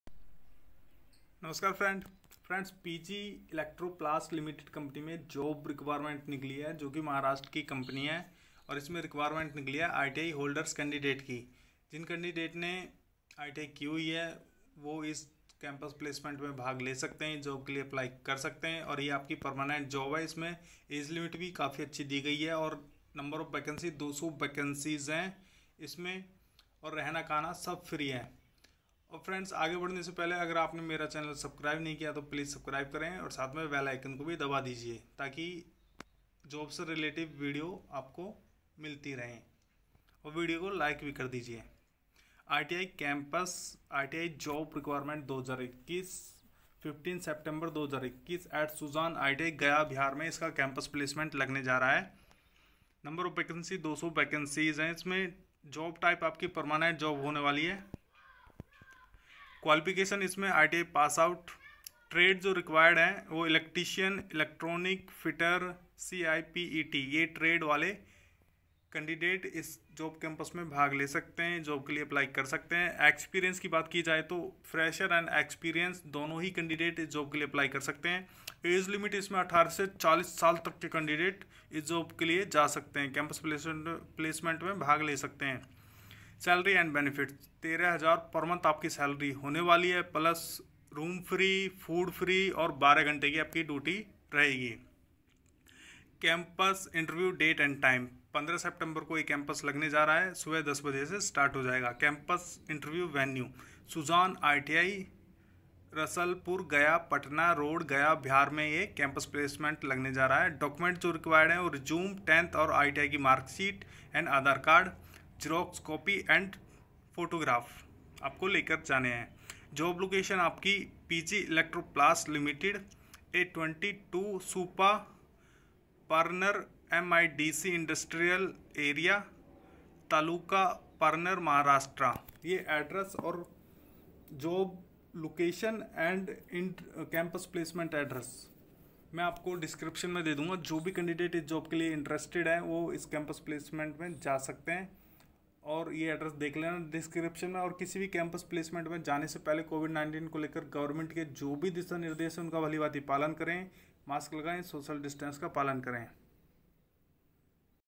Latest ITI Job Requirement | iti job requirement fresher | iti job in parner Maharashtra
Campus Interview Date and Time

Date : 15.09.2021
Time : 10:00am

Campus Interview Venue

Sujan ITI
Rasalpur, Gaya-Patna Road,
Gaya, Bihar 823003, India

Document Required
Resume, 10th & ITI Mark sheet,
 Aadhar Card Xerox Copy &
 2. Photograph

Job Location

PG ELECTROPLAST LTD

A 20/2, Supa Parner MIDC Industrial Area,
 Taluka, Parner, Maharashtra 414301, India